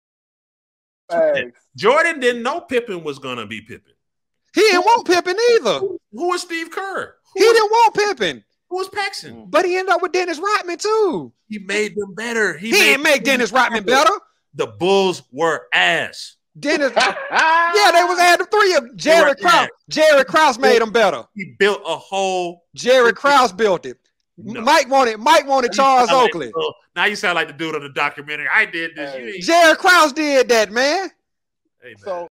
All Takes Matter feat DJ Scream-Who Built the Bulls Dynasty: Jordan or Jerry Krause?
All Takes Matter featuring DJ Scream discusses who built the Bulls Dynasty and who is to blame for players switching teams!